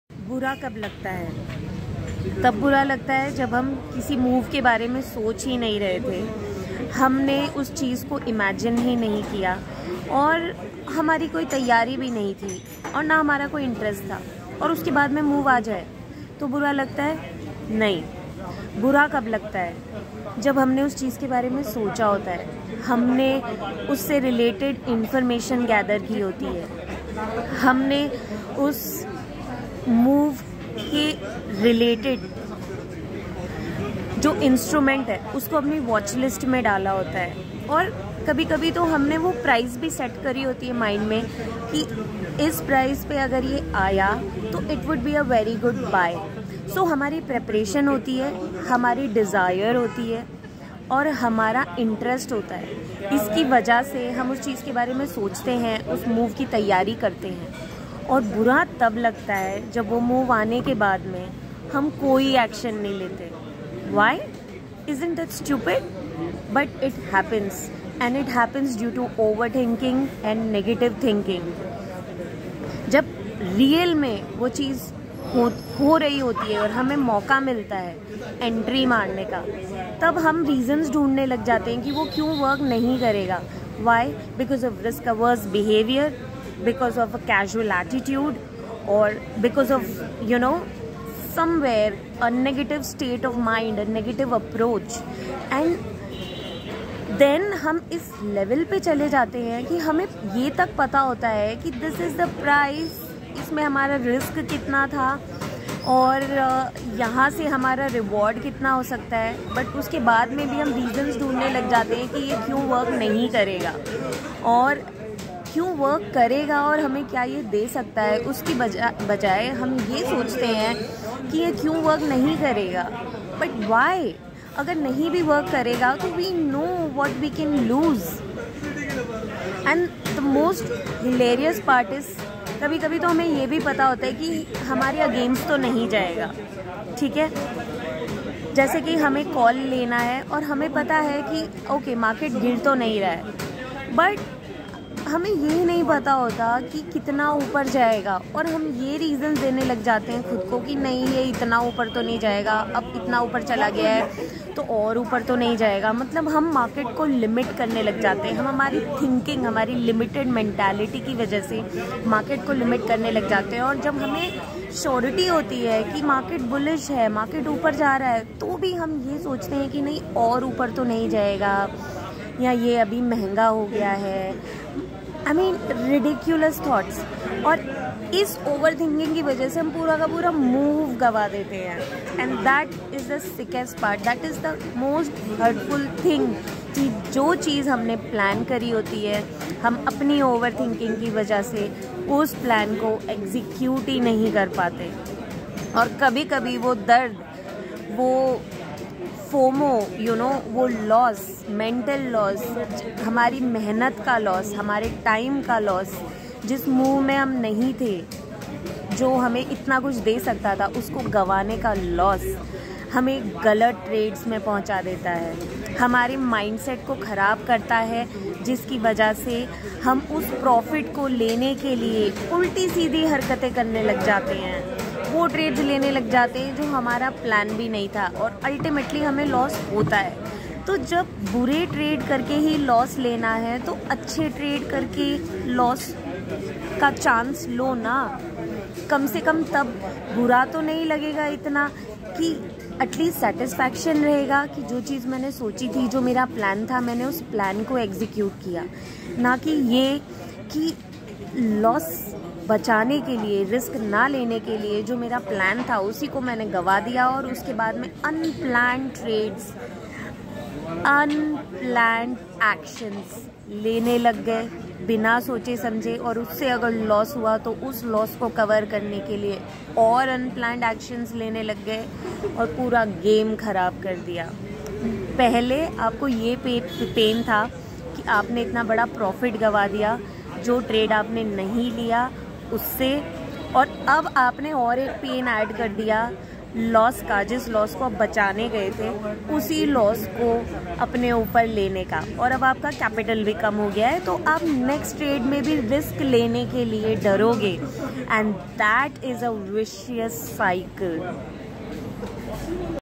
Why Trading is so hurtful for beginners/How risk averse behavior ultimately gives losses
In this video I have talked about the unreasonable stories we tell ourselves due to risk averse behavior and how it hurts the most when the move we anticipated comes but we could not jump in that opportunity due to overthinking and how that pain translates into losses.